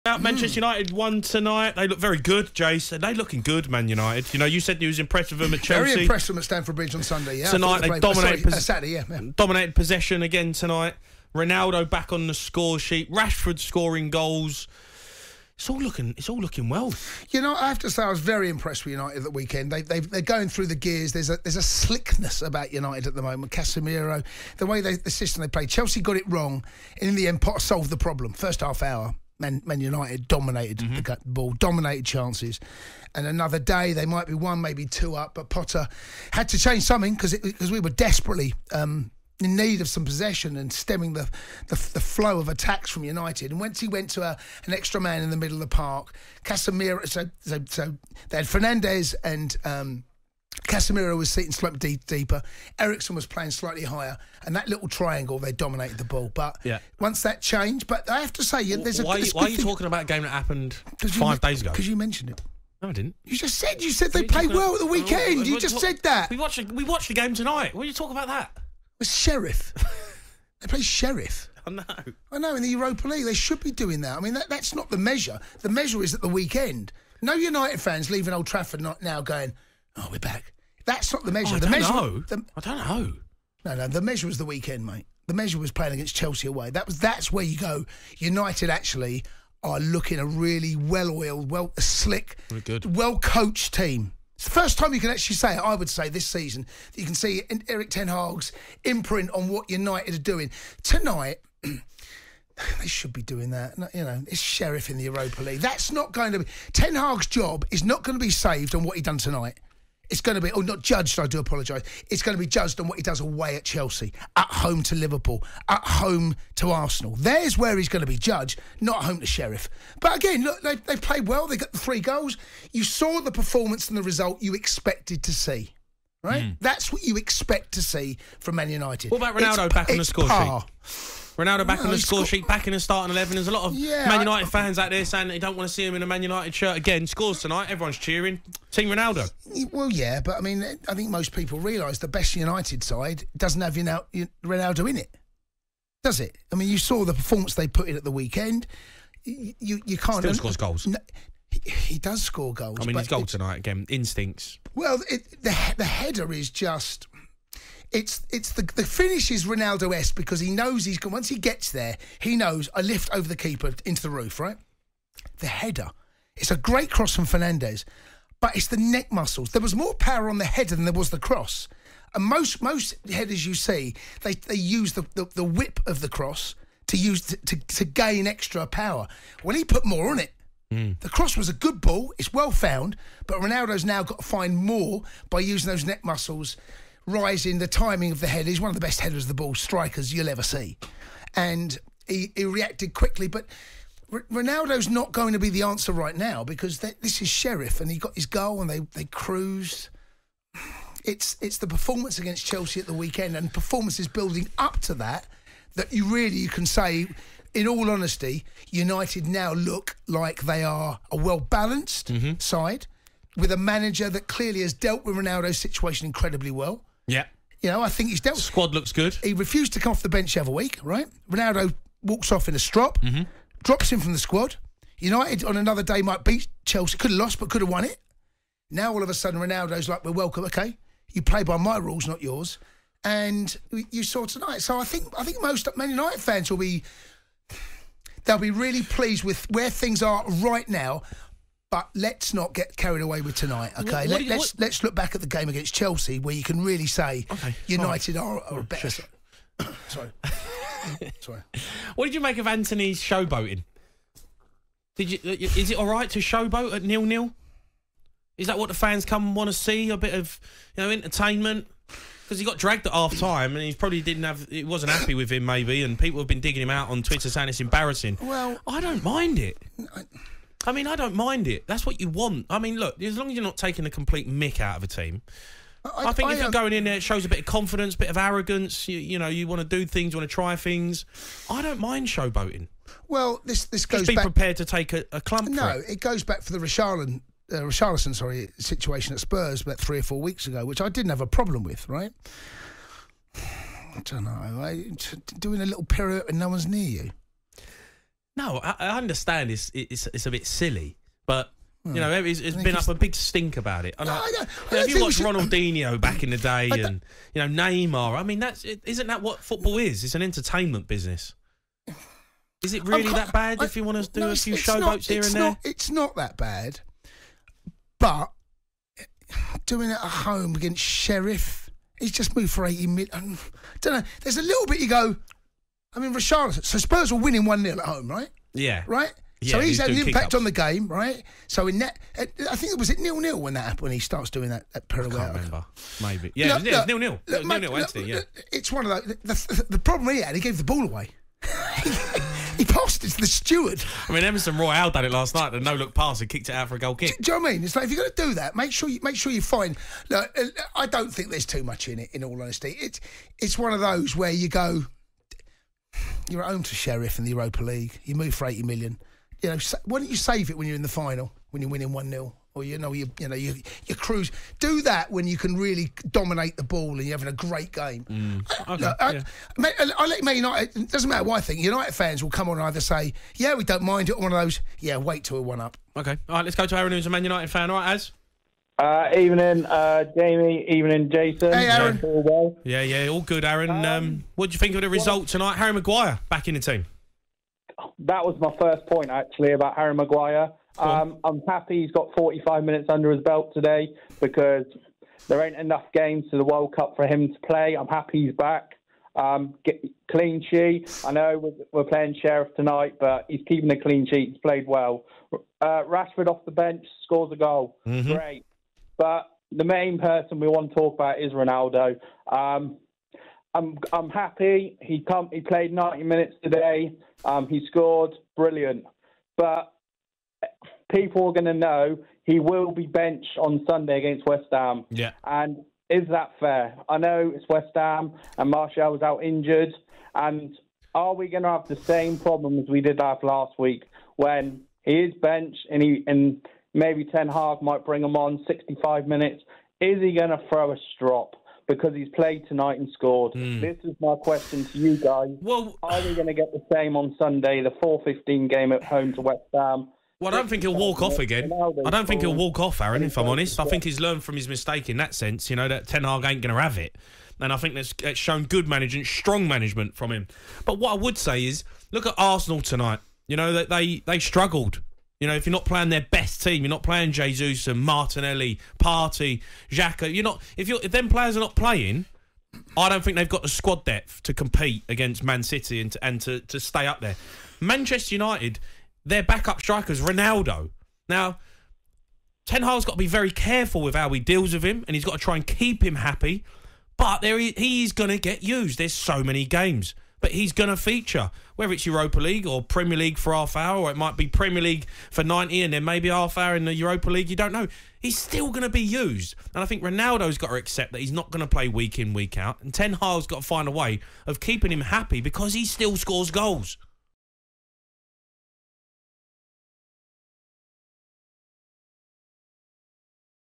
Jason Cundy & Jamie O'Hara react as Ronaldo returns to Man Utd's team with a goal! 🔥👀
Cristiano Ronaldo was out of Man Utd's last squad due to disciplinary issues but returned for their Europa League tie against Sheriff with a goal!